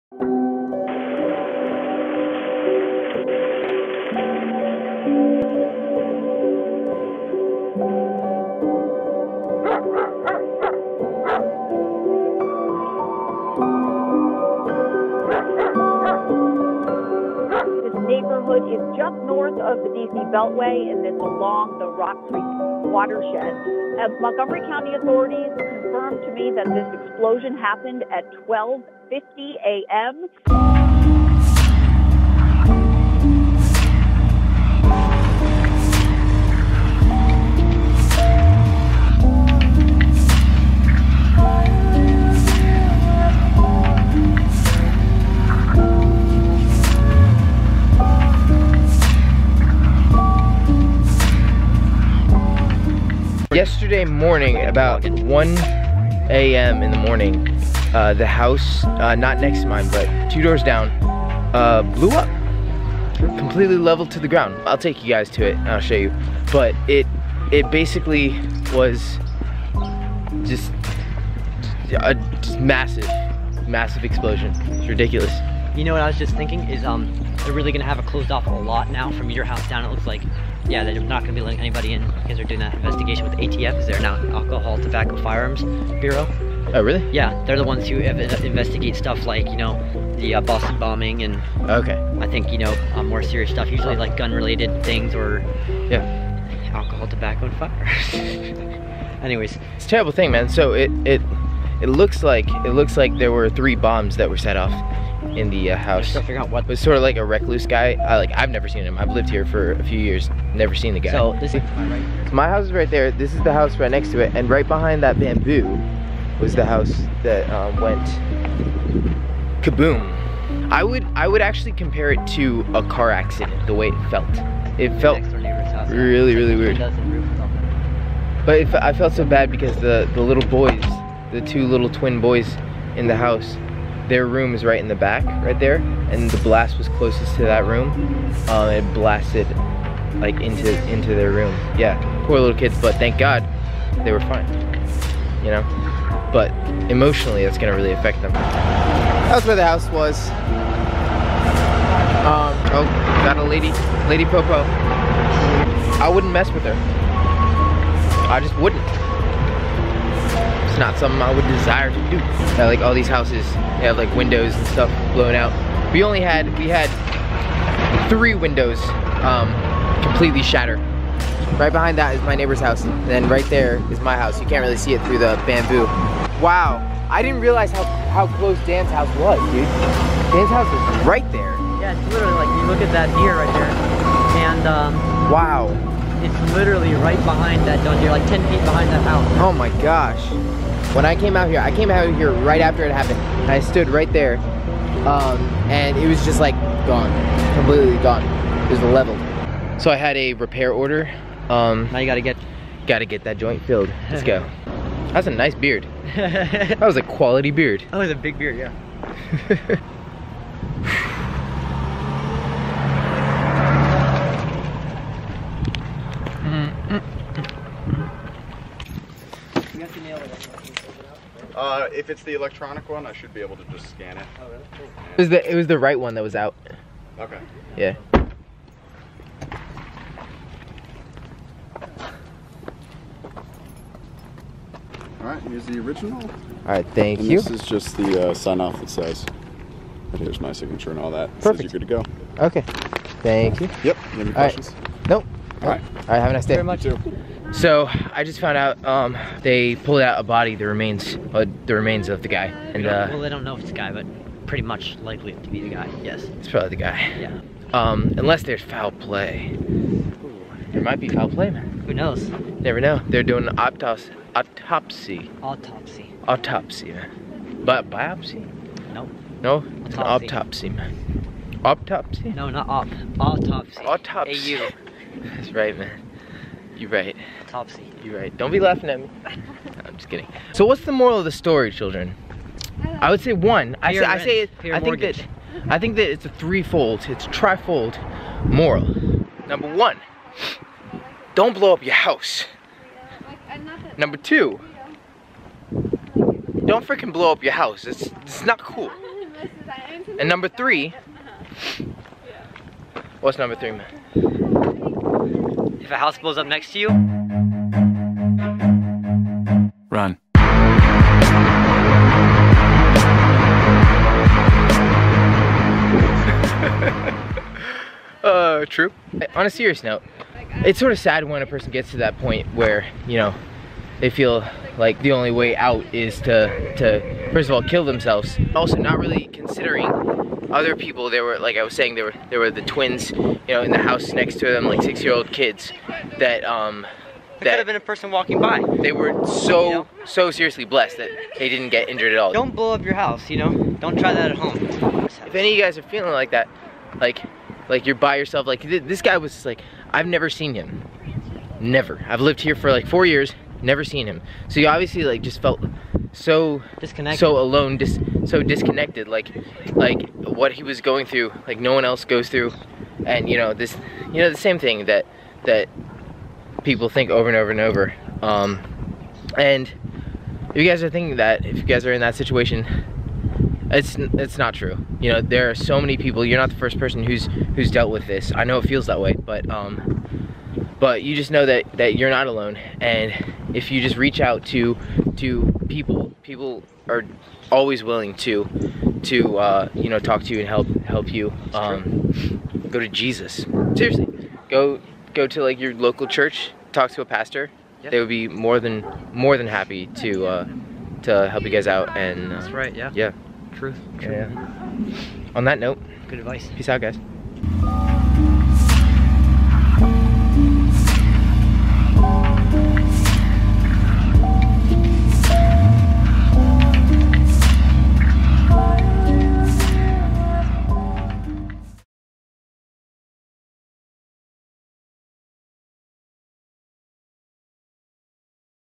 My Neighbors House Exploded!! (Rockville Home Distroyed in Explosion)
So at 1:00 a.m. on March 17th. the house two doors down from me was completely leveled in a thunderous explosion audible for miles around. Many of the homes on either side were also damaged. The remains of the occupant were found, and the results of the autopsy have not yet been made public.The explosion brought 75 firefighters to the scene due to the severity of the blast and aftermath. In the weeks following, my next-door neighbor's (closest to the blast) house has been condemned, and they are living elsewise wile repairs are being made. I ask you all for your prayers for his family and for everyone a affected by this terrible event.

Make sure to check out my friend over at CW.Volgs to see his perspective on this crazy event.